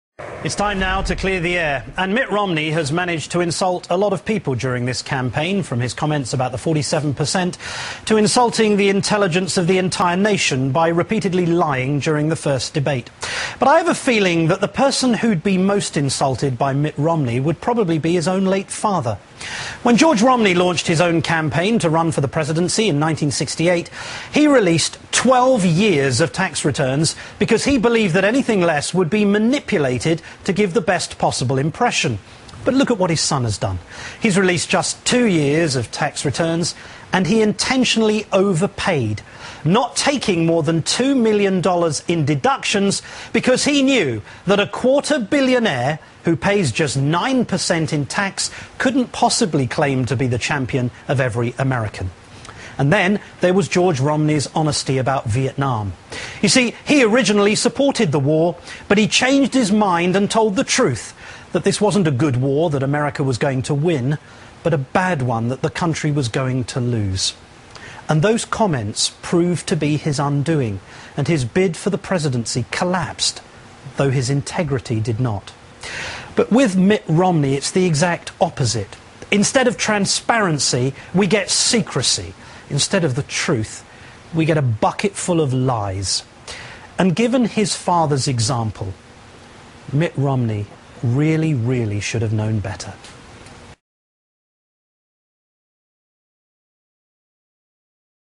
mitt romney's Debate Fail and Lies Revealed - October 5, 2012
Unelectable liar and welfare bum mitt romney set up his own scattered mine field in the first of three debates with his and Our President Barack Obama, and now incomparable liar and buffoon mitt romney has to walk back through his mine field, wearing a blindfold and being kicked from behind by President Barack Obama, who let mitt bury himself in multiple and devastating lies that are well known to the American voters. mitt has buried himself in an intractable situation, and now stands a 0% chance of slithering in to Our White House, which will continue to be occupied by Our President Barack Obama for over four more years. Goodbye mitt you lying degenerate.